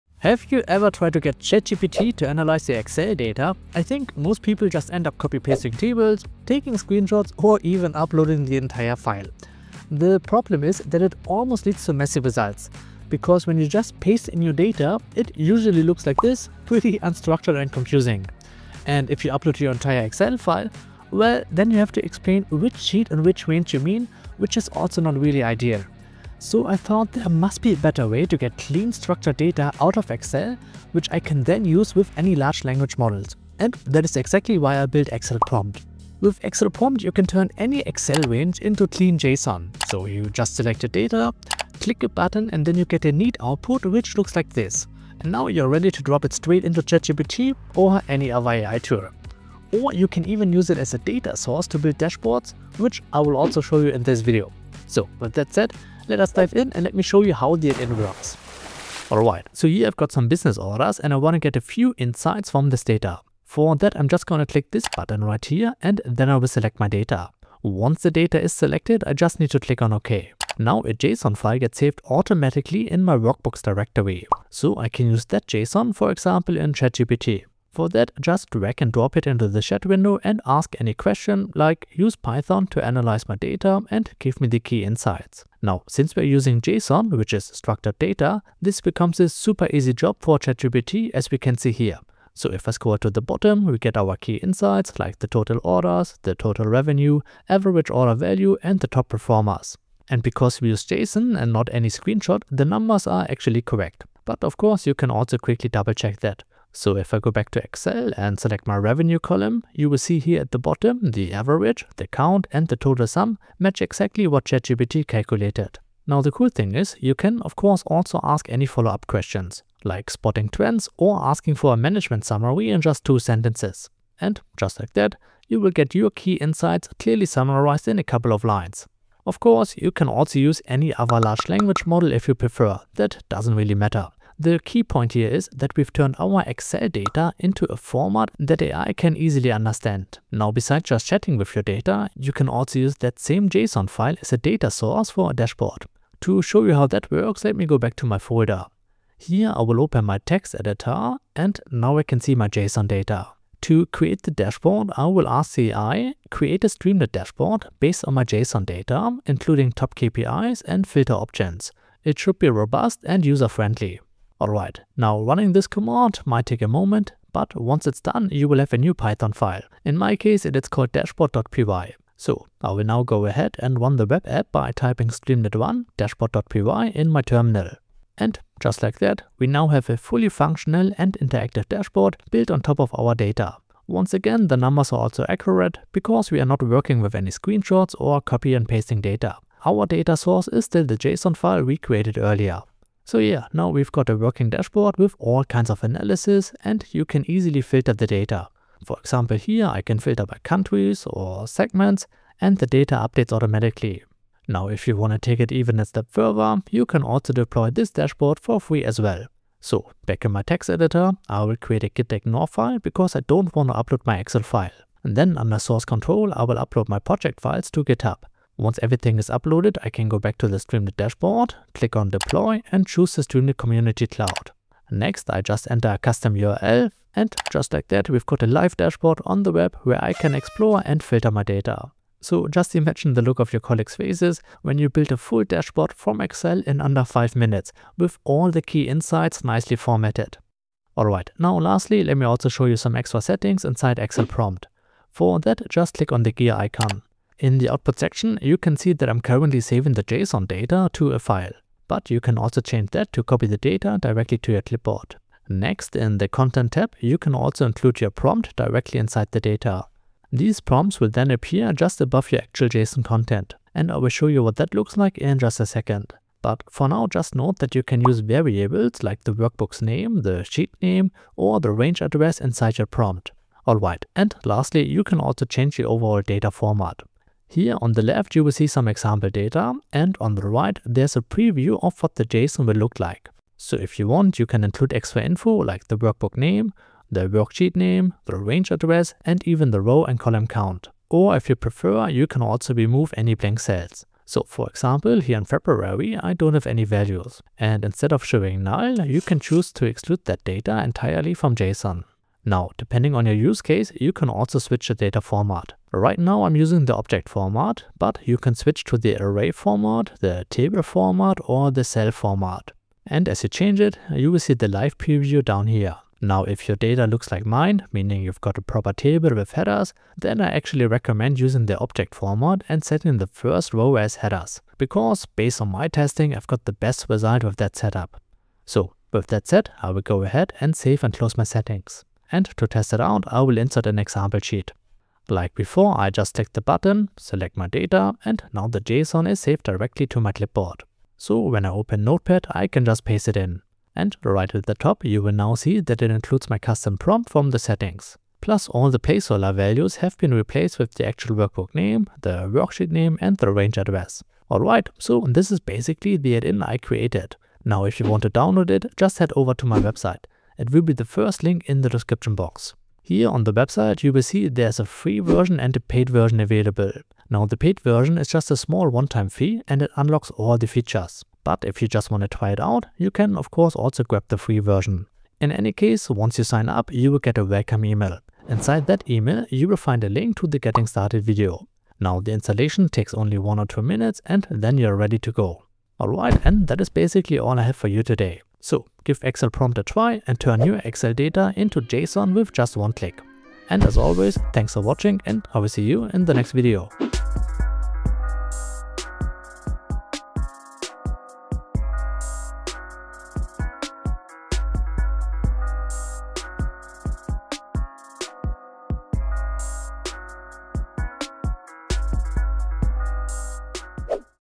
How to Turn Your Excel Data into Usable AI Data (JSON)
𝗗𝗘𝗦𝗖𝗥𝗜𝗣𝗧𝗜𝗢𝗡
In this video, I show you how to turn any Excel range into clean JSON with just a click, using XLPrompt, my free Excel add-in. You’ll see how to export your Excel data in the right format so you can use it with ChatGPT or any other AI tool. No more copy-pasting, screenshots, or messy results. XLPrompt makes it easy to get structured data out of Excel, fast. You can also use this JSON as a data source for dashboards or other automations.

⭐ 𝗧𝗜𝗠𝗘𝗦𝗧𝗔𝗠𝗣𝗦:
00:00 – Introduction
00:58 – Convert Excel to JSON
02:20 – Create Interactive Dashboard
04:00 – XLPrompt Settings
05:57 – Download XLPrompt
06:34 – Outro